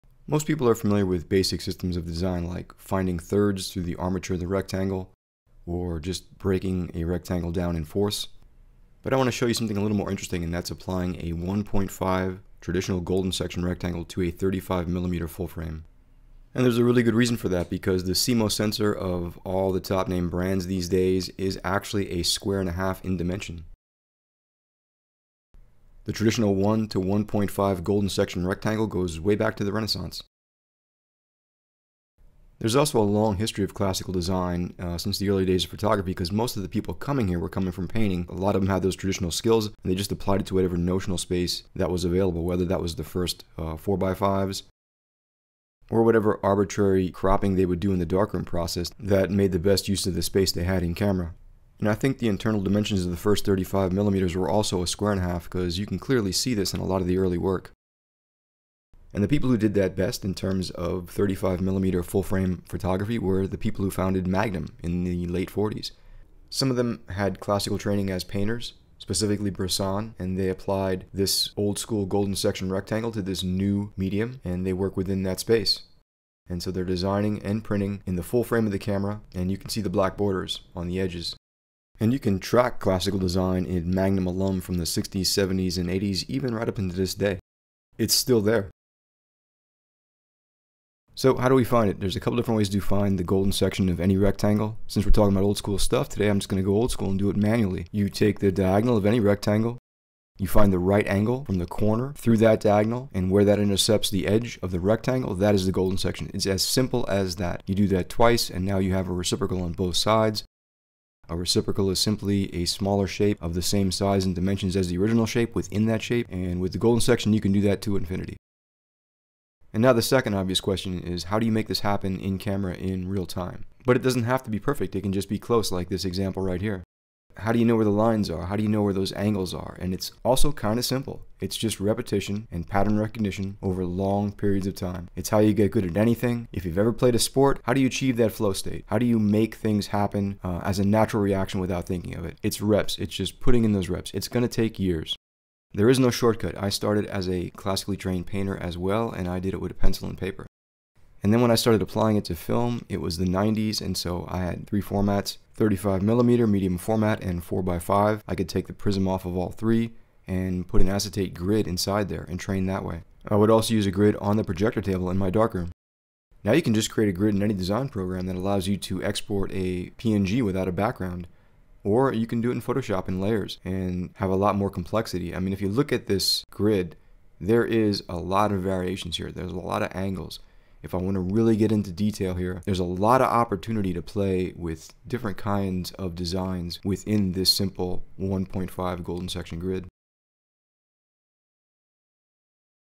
Designing in the 35mm full frame
how to design in the dimensions of a 35mm full frame using a 1.5 golden section rectangle
0:08 golden section
0:16 CMOS sensors
0:25 early photo design
0:58 Magnum
1:29 create a 1.5
1:58 application
2:45 complexity = opportunity